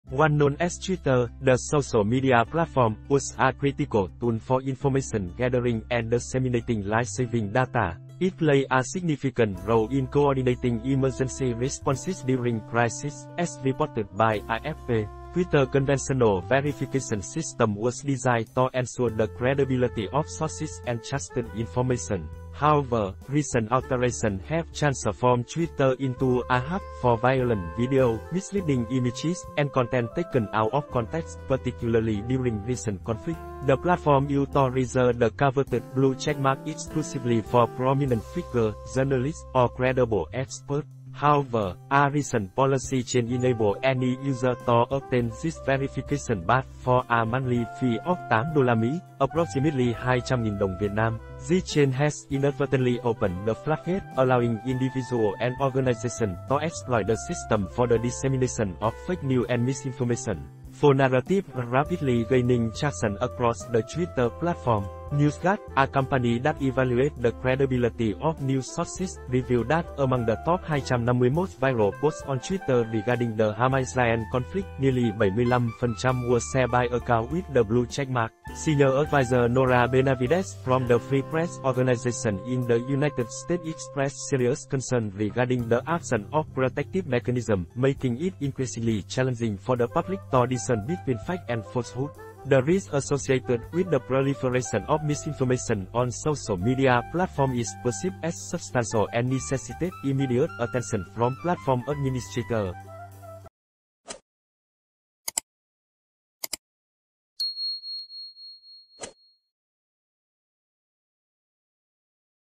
TWITTER'S VERIFICATION SYSTEM AND THE SPREAD OF MISINFORMATION
Once known as Twitter, the social media platform, was a critical tool for information gathering and disseminating life-saving data. It played a significant role in coordinating emergency responses during crises, as reported by AFP. Twitter's conventional verification system was designed to ensure the credibility of sources and trusted information. However, recent alterations have transformed Twitter into a hub for violent videos, misleading images, and content taken out of context, particularly during recent conflicts. The platform used to reserve the coveted blue checkmark exclusively for prominent figures, journalists, or credible experts. However, a recent policy change enables any user to obtain this verification badge for a monthly fee of $8 (approximately 200,000 VND). This change has inadvertently opened the floodgates, allowing individuals and organizations to exploit the system for the dissemination of fake news and misinformation. False narratives are rapidly gaining traction across the Twitter platform. NewsGuard, a company that evaluates the credibility of news sources, revealed that among the top 250 most viral posts on Twitter regarding the Hamas-Israel conflict, nearly 75% were shared by accounts with the blue checkmark. Senior advisor Nora Benavidez from the Free Press organization in the United States expressed serious concerns regarding the absence of protective mechanisms, making it increasingly challenging for the public to discern between facts and falsehoods. The risk associated with the proliferation of misinformation on social media platforms is perceived as substantial and necessitates immediate attention from platform administrators.